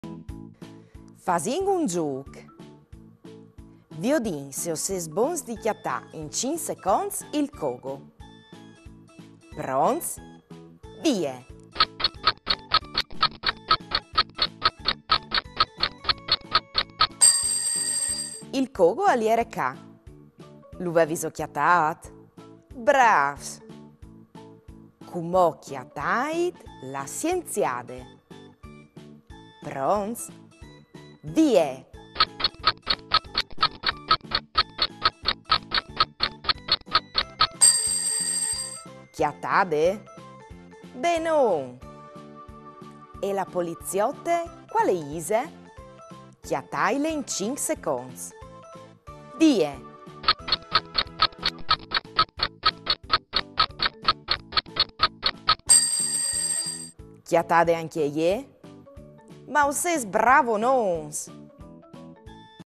146. Fasìn un zûc - I mistîrs
Tra i personaçs intal dissen cjatait chel che us domande Veronica
Tra i personaggi nel disegno trovate quello che vi chiede Veronica